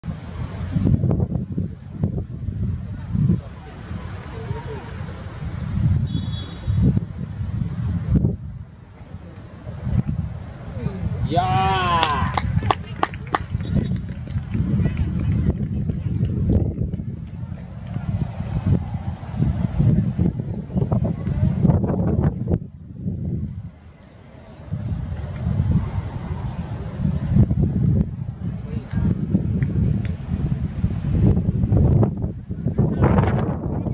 2016 東海地域リーグU-15 清水エスパルスJr.ユースvsジュビロ磐田U-15 2点目
■エスパルス得点者
川本 梨誉
青島 健大←

■エスパルスメンバー
石井 飛雄馬
鈴木 瑞生
西島 隆斗
東海林 泰地
林 航輝
川本 梨誉
丸山 優太朗
佐野 由尚（→成岡 輝瑠）
江本 颯汰
古長谷 千博（→青島 健大）
青島 太一